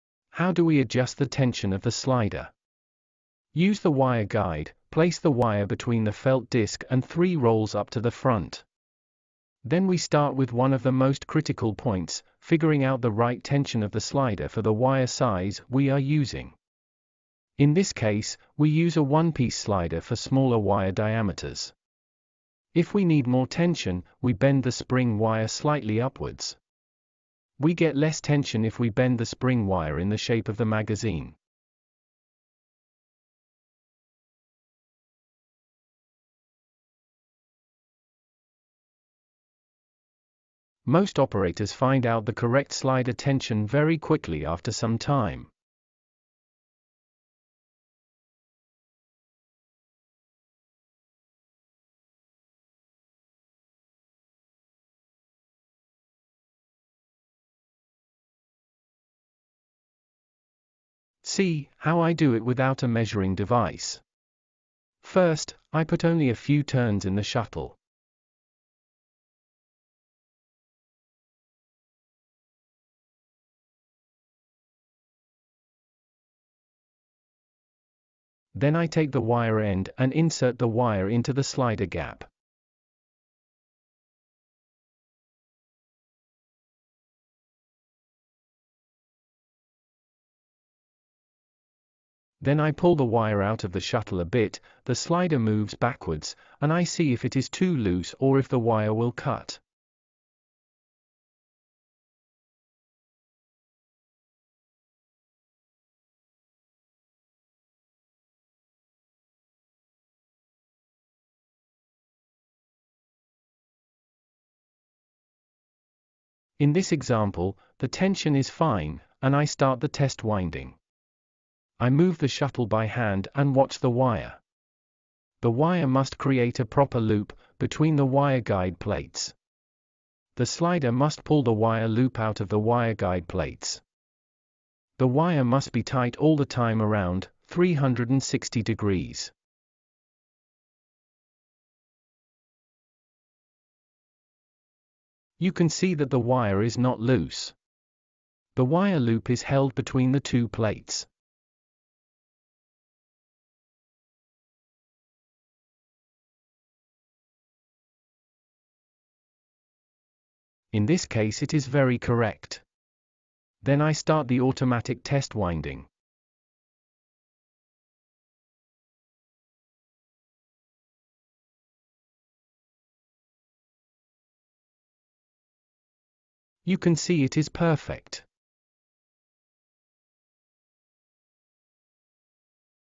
Toroidal winding machine setup the slider tension
Toroidal winding machine TW100S from Credo-tech.de. How we can setup the slider tension.